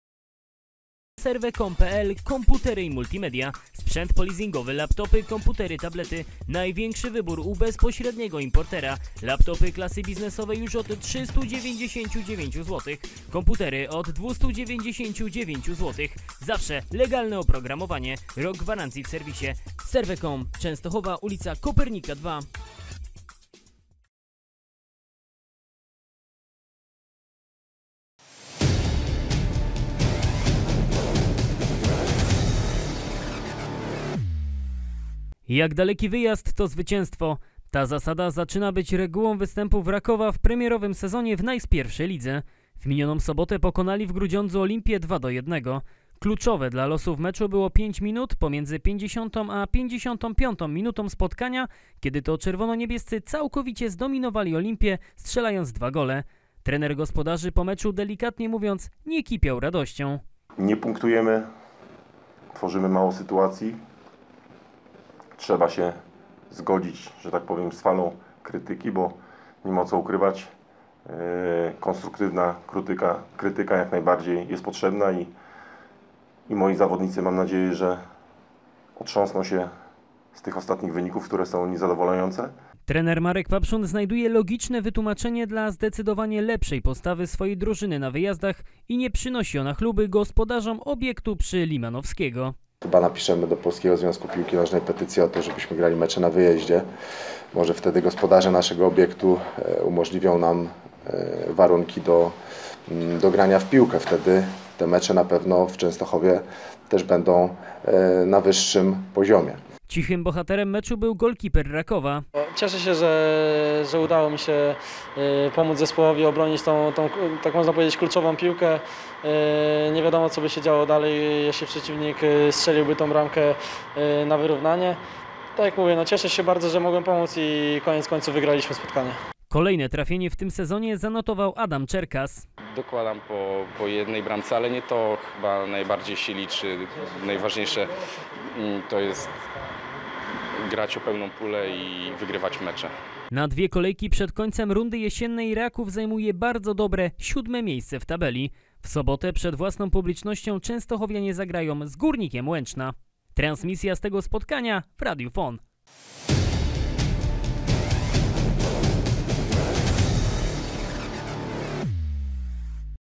Czy Raków napisze petycję do PZPN-u o granie na wyjazdach? - VIDEO
Z tarczą powrócili z Grudziądza piłkarze Rakowa Częstochowa. Wygrana umocniła czerwono-niebieskich w górnych rejonach tabeli. Na pomeczowej konferencji prasowej trener Marek Papszun znalazł bardzo logiczne wytłumaczenie lepszej postawy zespołu na wyjazdach i niestety… nie napawa ono optymizmem na najbliższą przyszłość. Więcej w materiale wideo.